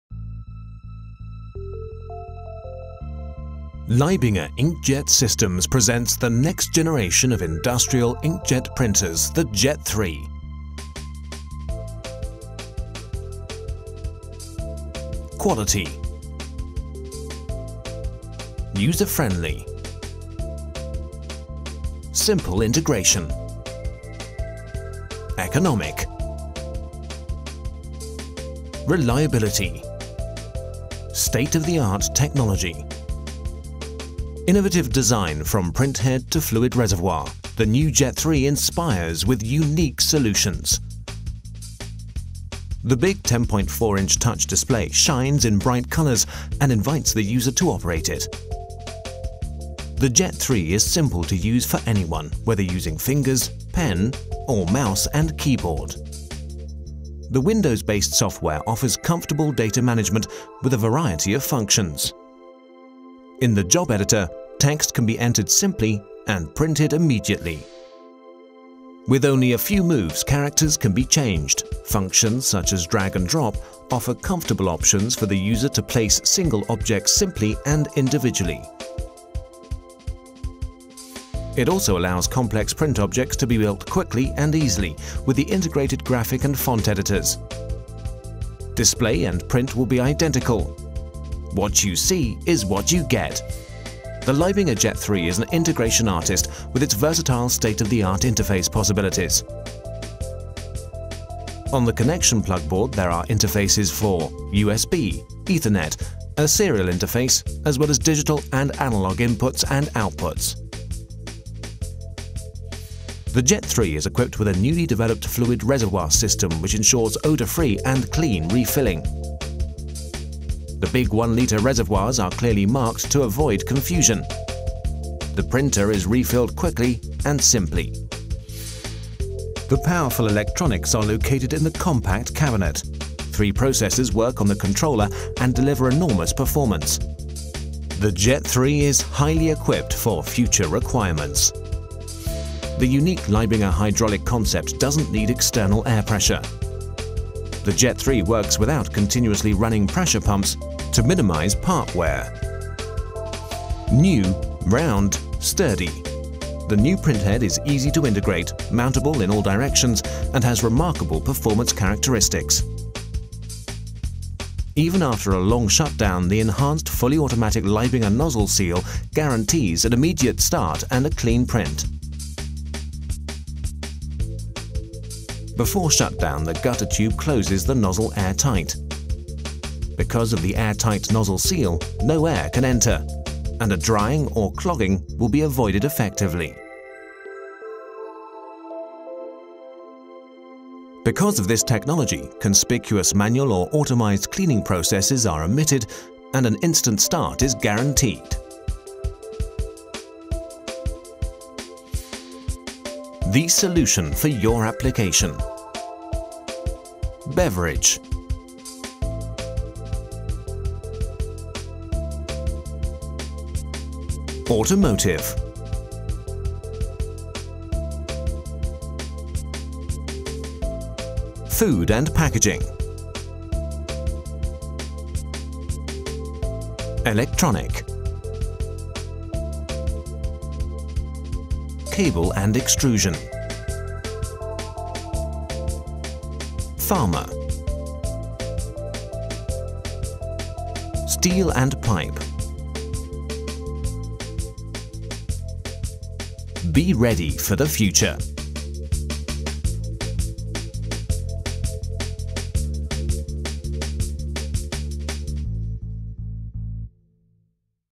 Small character inkjet printer LEIBINGER-JET3
JET3

The professional inkjet printer among the industrial inkjet printers. The LEIBINGER JET3 offers intuitive and comfortable operation based on Windows CE Technologie® using a 10,4" colour TFT touch display. Large keys and menu navigation in different languages as well as endless numbers of special functions like graphic and font editor make the handling extremely easy. The newly developed refill system guarantees a clean and splash-free and smell-free refilling process.

Specifications:

Printing speed: Up to 6,6 m/s (10 cpi)

Inks
- Different dye-based and soft-pigmented inks
- Fast drying inks
- MEK, Keton free, alcohol based inks
- Temperature and transfer resistant inks
- Security, colour changing, sterilization, adhesive and alcohol resistant inks fluorescent
- Food grade ink and Food packaging inks
- Customized inks

Ink colours: Black, yellow, red, orange, white, blue, green

Environmental conditions and Supplies:
- 100 V-240 V, 50-60 Hz, max. 40 VA
- Temperature range +5 °C to +45°C
- Relative humidity max. 90% non condensing
- No external supply of compressed air is required
- Cabinet according to class IP 54

Amount drops: 32 Drops